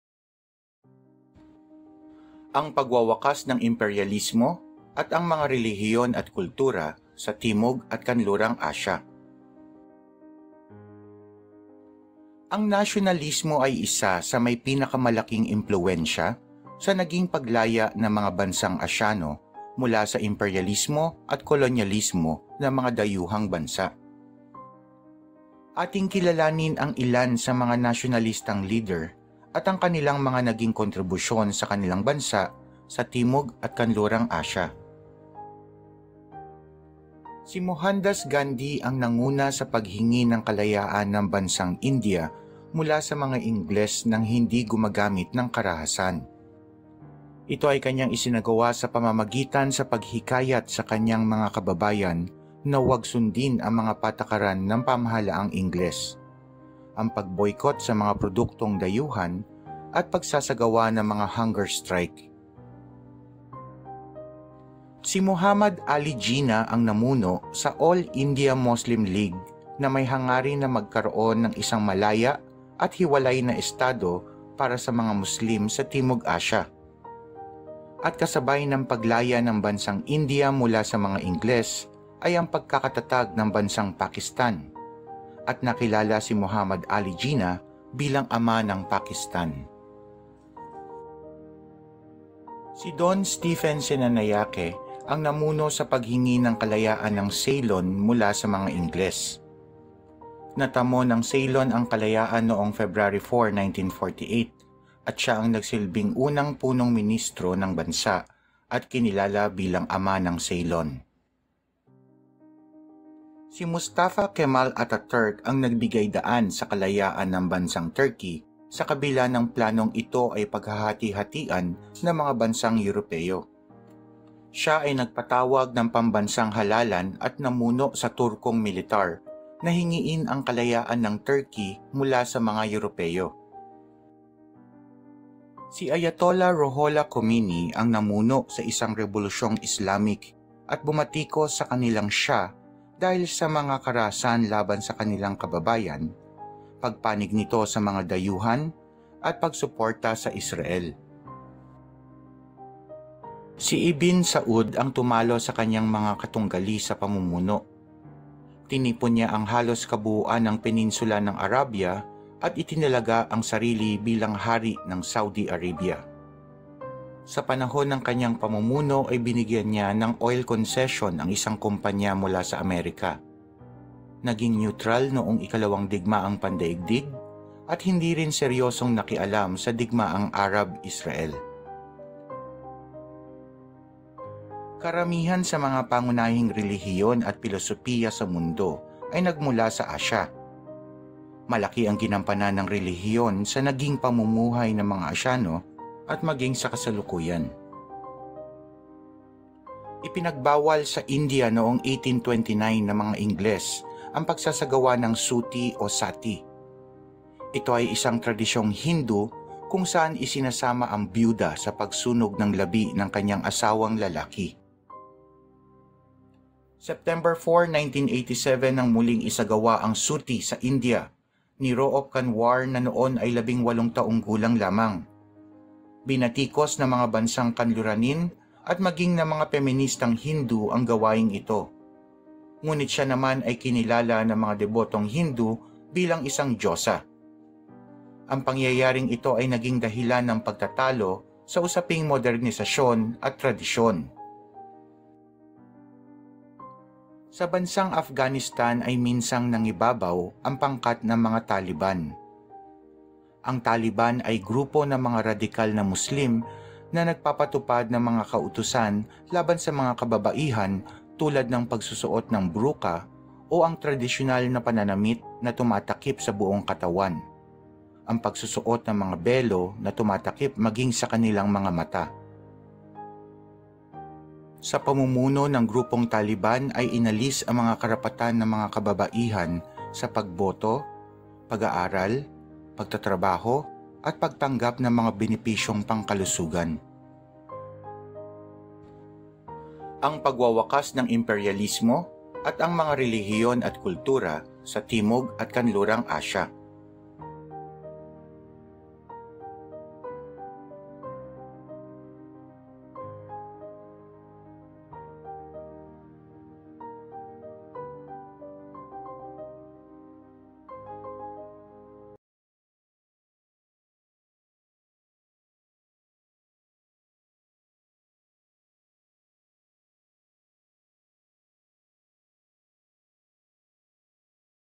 Pagwawakas ng Imperyalismo at mga Relihiyon at Kultura sa Asya
Ang bidyong ito ay nilikha upang makatulong sa pagkatuto ng mga
mag-aaral sa Araling Panlipunan 7: Araling Asyano

0:00 Intro
0:36 Mohandas Gandhi
1:04 Mohammad Ali Jinnah
1:32 Son Stephen Senanayake
1:53 Mustafa Kemal Atataturk
2:15 Ayatolla Rouhollah Khomeini
2:33 Ibn Saud
3:04 Relihiyon at Kultura ng Asya
3:19 Suttee sa India
4:06 Taliban sa Afghanistan
4:49 End

You can send support thru my GCASH # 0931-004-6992